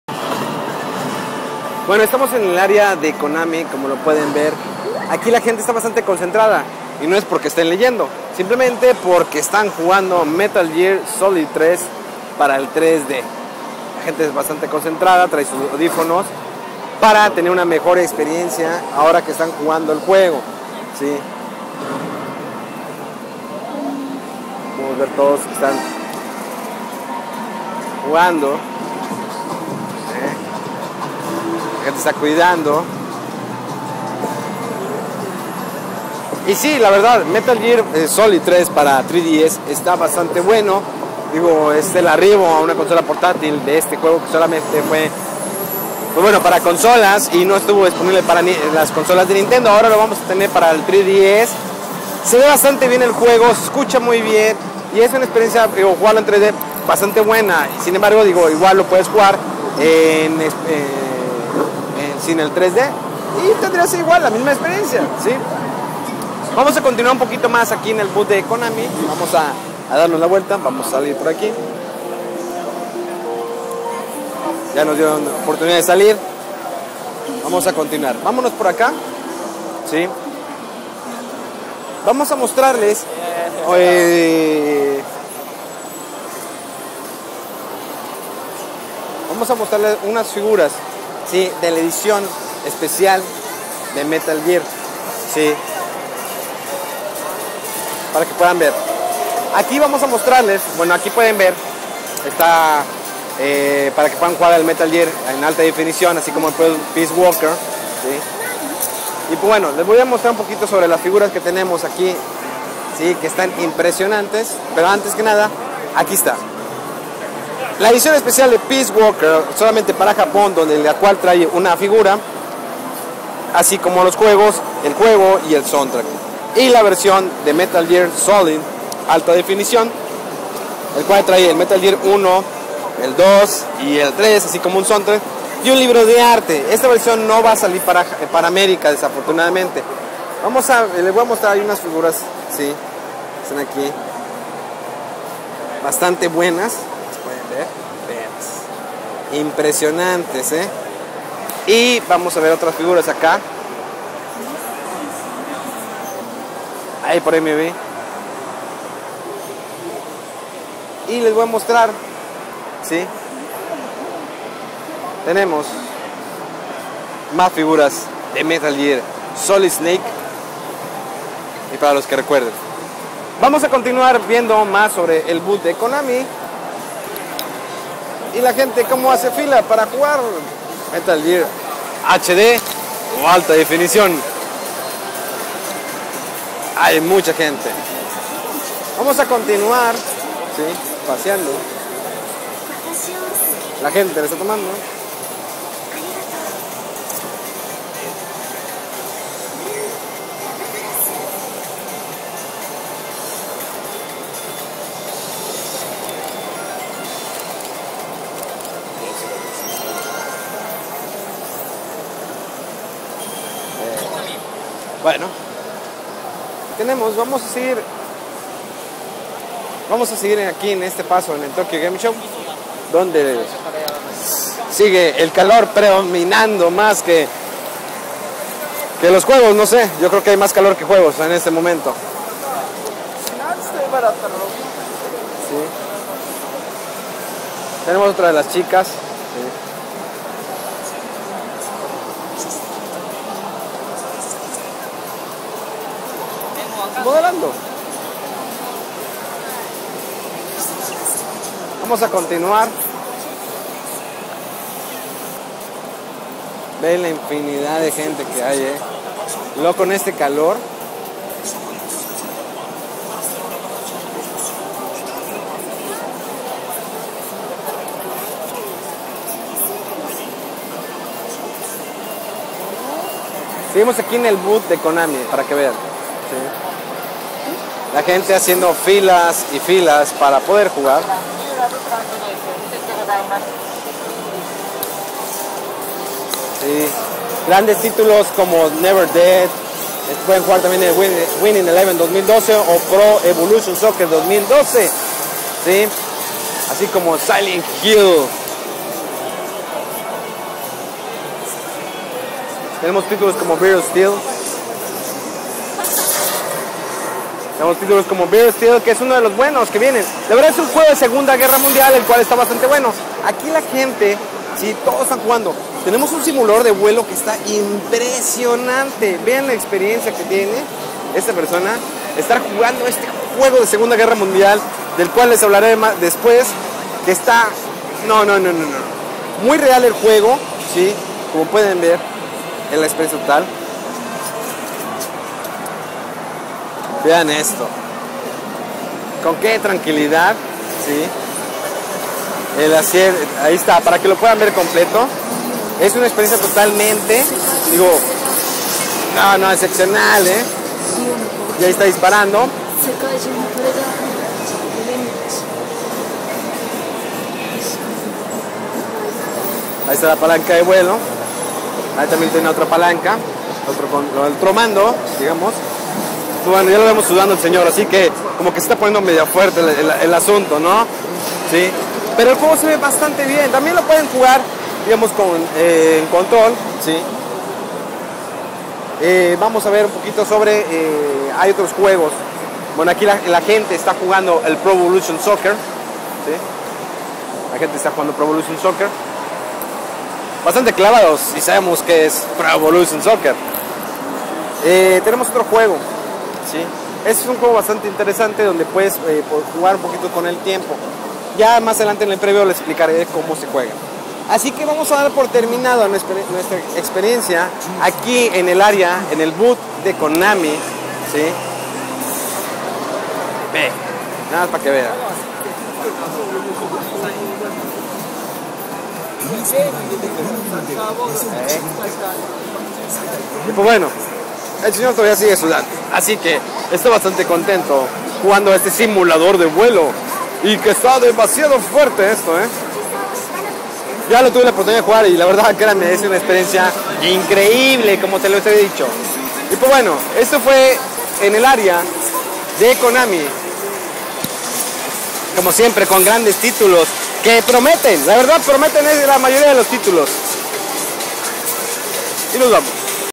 Konami on Tokyo Game Show 2011
vistiando el Booth de Konami en el Tokyo Game Show 2011